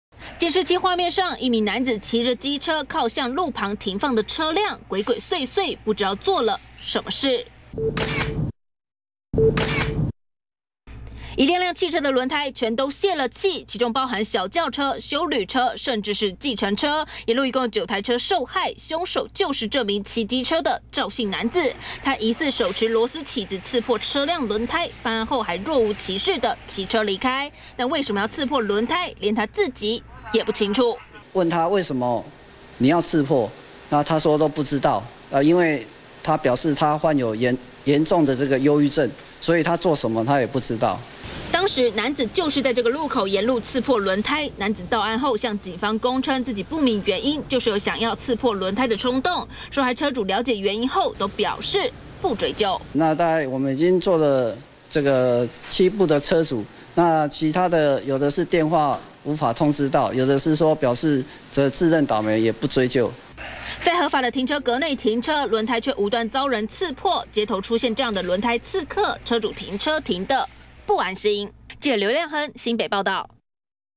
20151210中天新聞 「刺客」！男深夜持螺絲起子 刺破路旁輪胎
新北市新店有車主發現車輛停放路邊，輪胎卻無故遭刺破，報警處理，警方調閱監視，發現是一名男子手持螺絲起子犯案，男子到案後供稱，就是想要「刺破輪胎」。